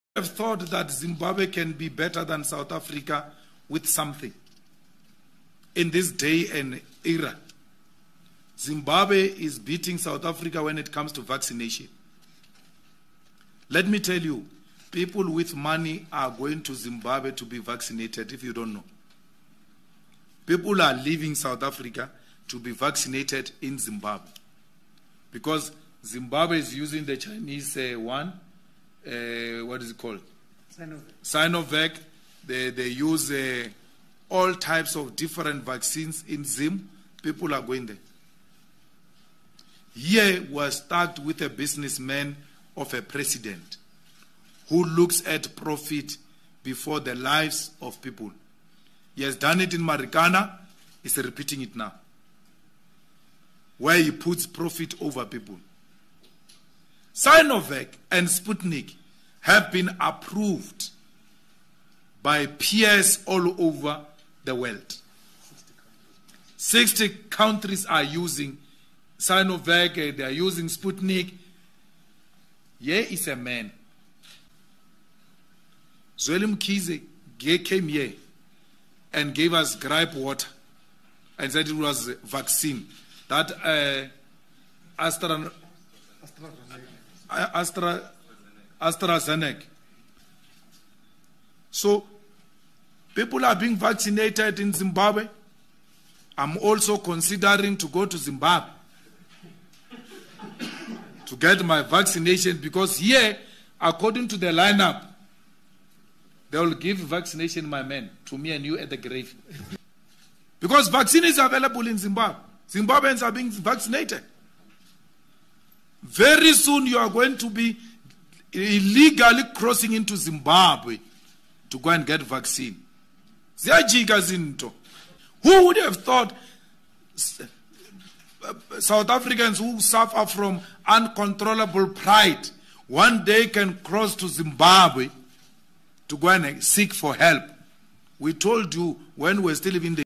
South Africans crossing the border to get COVID-19 vaccine in Zimbabwe
EFF President Malema says rich South Africans are now crossing into neighbouring Zimbabwe get vaccinated against Covid-19